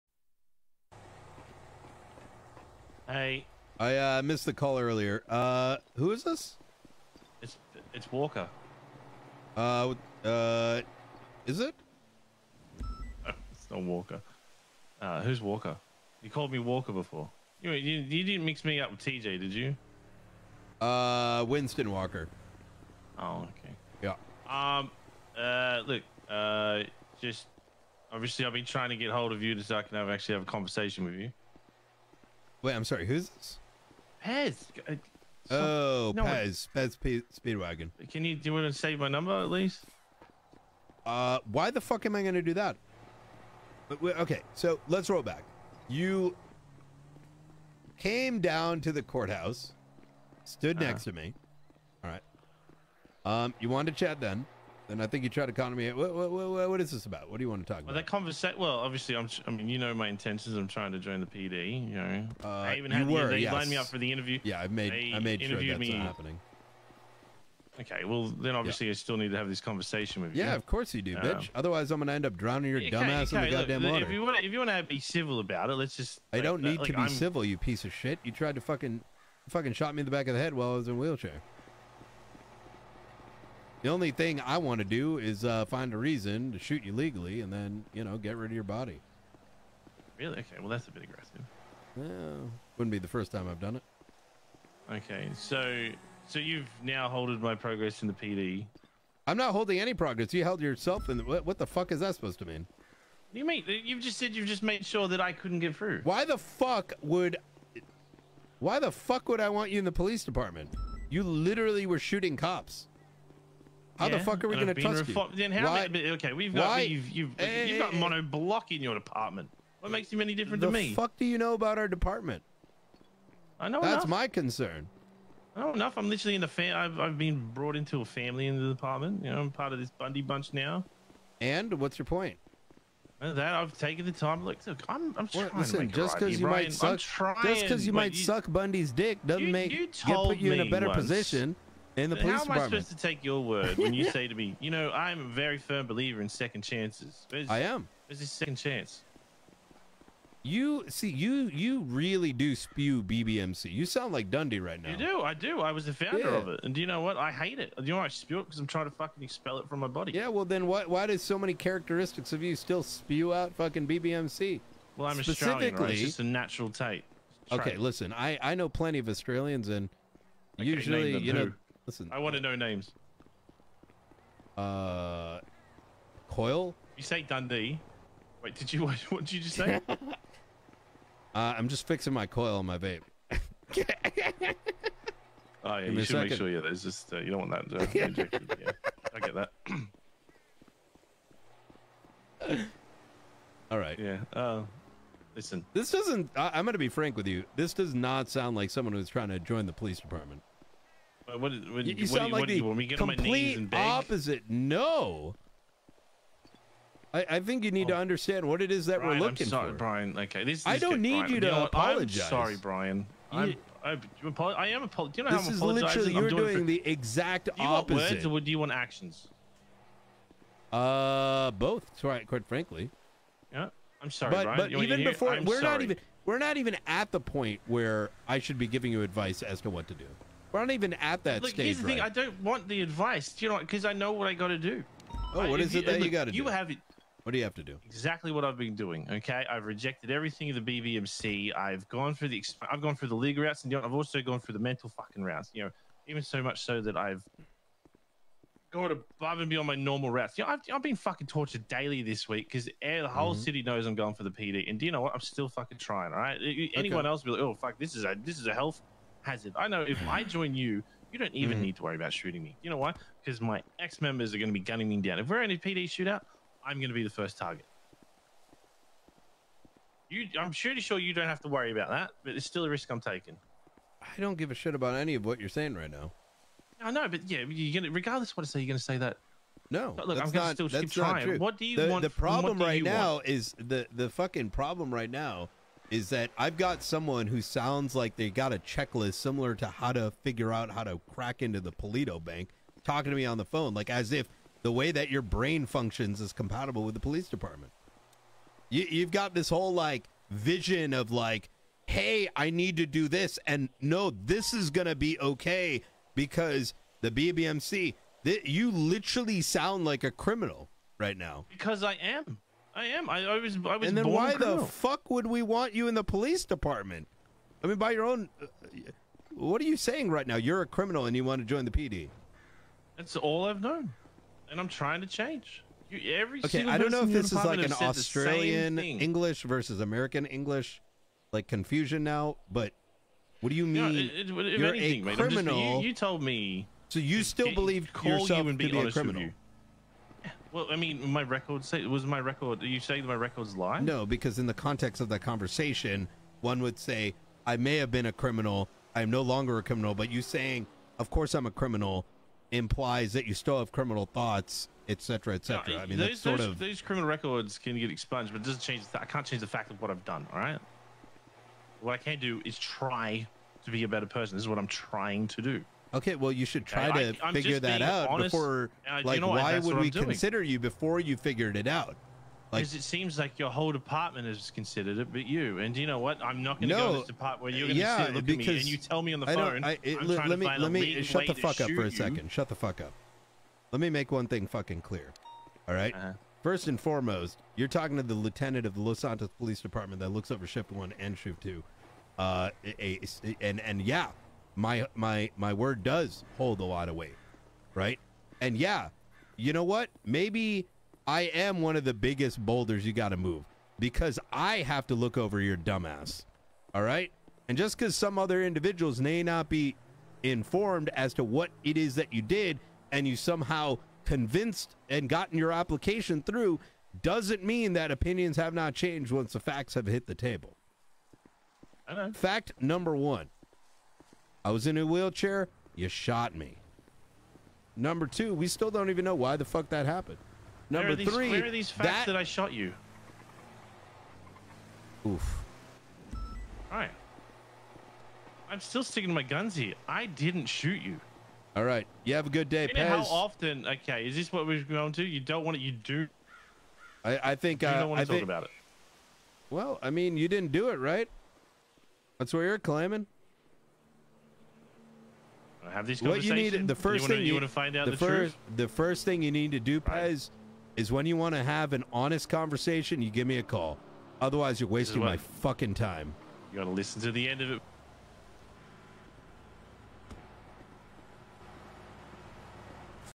Life lesson : What a manipulator trying to get what they wants look like
In this exchange, Pez/Cody Speedwagon is trying to get Brian Knight to tell him what he wants to see so that Brian will let him into PD.

This is a great example of a manipulative person trying to pry for information about how you can be manipulated into believing that they have changed in order to get what they want from you. Their intent is not genuine change. Their intent is to make a display of change just to convince you to give them what they want or rather, to remove barriers to you giving them what they want. Think "Fake it 'til you believe it" or "Fake it 'til you've run out of reasons".

Engaging into this mind game will put you at a disadvantage, as later down the line, you will be backed into a corner when the manipulator has fulfilled your checklist and you have run out of reasons not to give them what they want. You will then be much more vulnerable to getting "convinced" to give them what they want and when you don't, you will be made to feel irrational and unreasonable about your decision.

Thankfully, Brian sees what Pez is up to ; Pez trying to befriend him and get close to him for no valid reason (other than needing something from him), Pez trying to get Brian to be friendly to him, Pez talking to other people about him instead of talking to him directly (dispatching agents to speak on his behalf) and then this. Instead of getting swept into Pez' mind games, he masterfully shuts down all of it and gives him nothing. He knows that the only winning move against a manipulator is not to play.